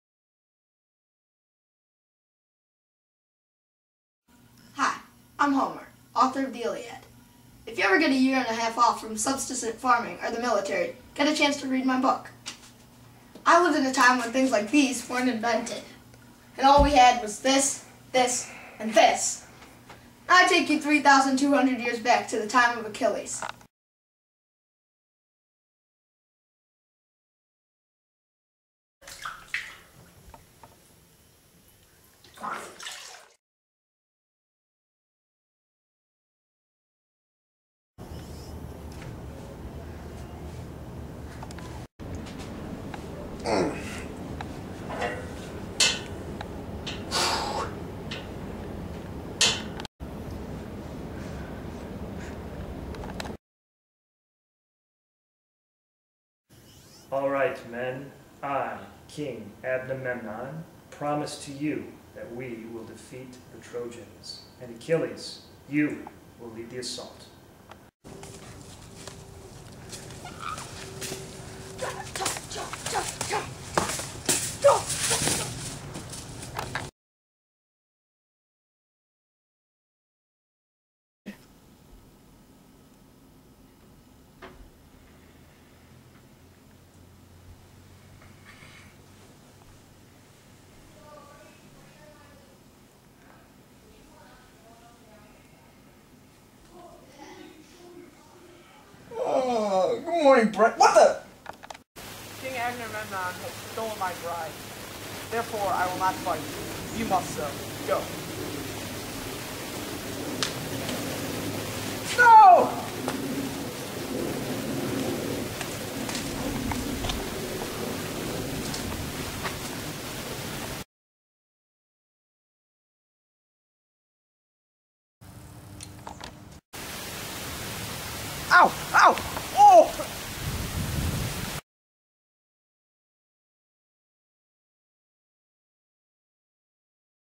Achilles Movie
Jake Ian and I made this movie for mine and ian's project. I thought that it was funny.